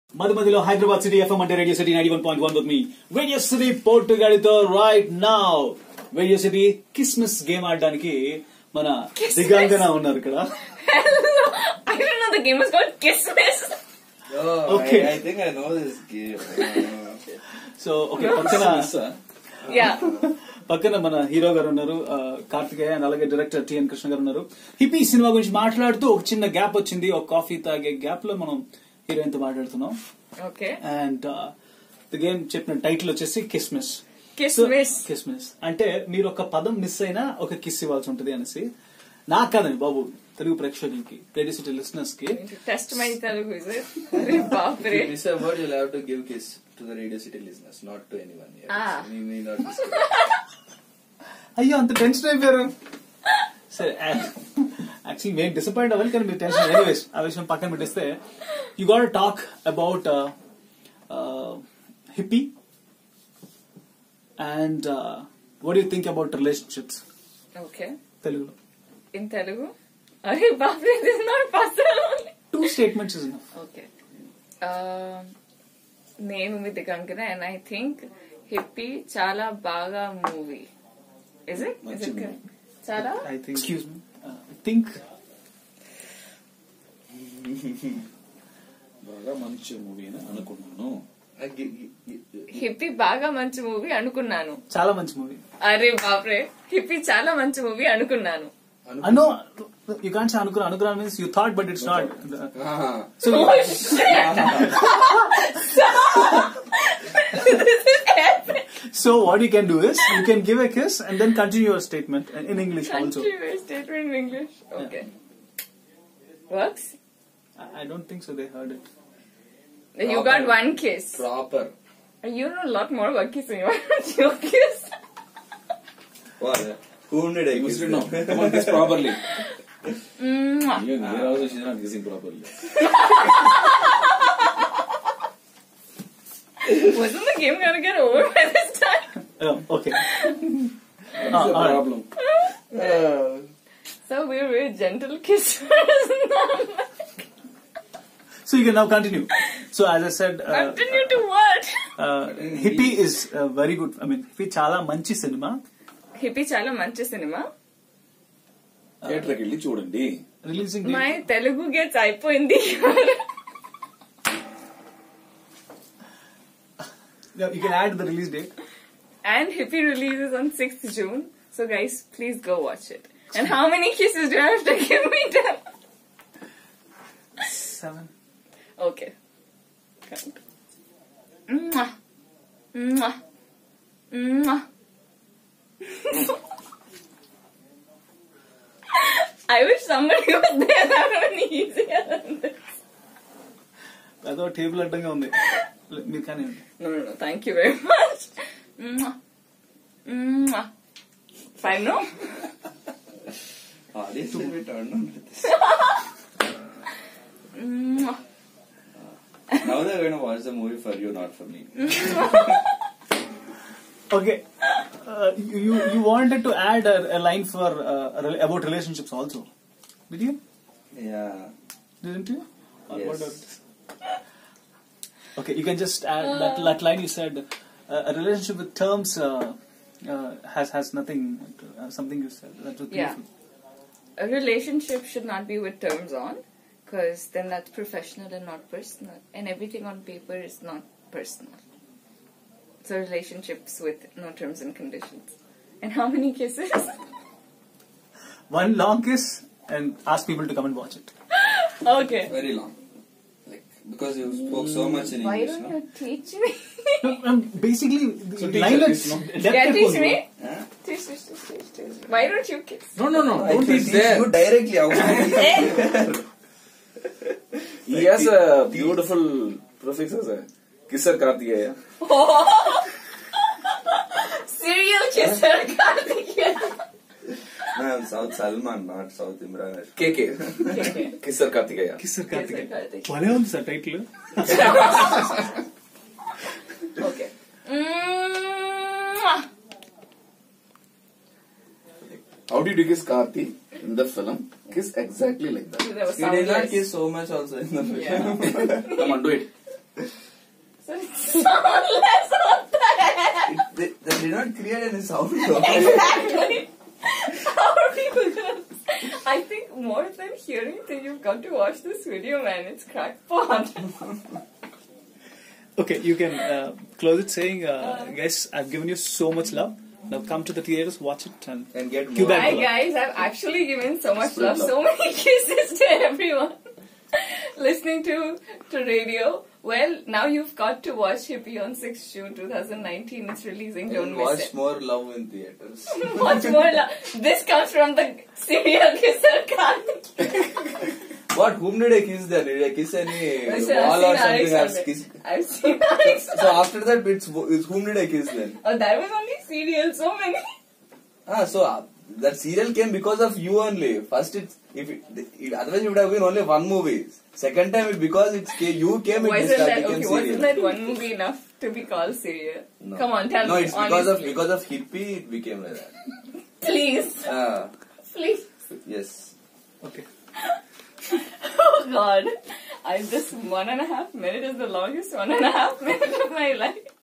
The Kiss Game with Hippi Heroine Digangana Suryavanshi | RJ Potugadu | Star Express Telugu
Watch this fun-filled Kiss game Video with RJ Potugadu and Digangana Suryavanshi.